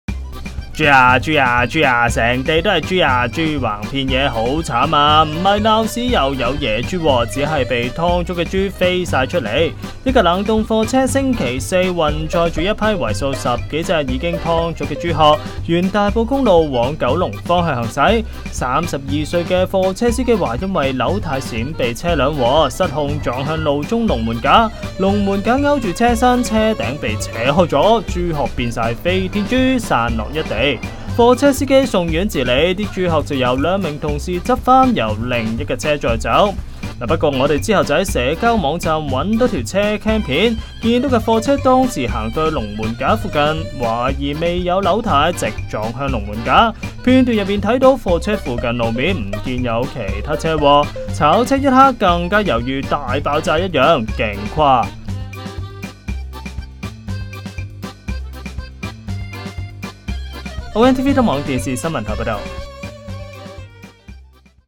車CAM直擊貨車炒車 豬橫遍野！
大埔公路貨車撼龍門架，豬殼散布馬路。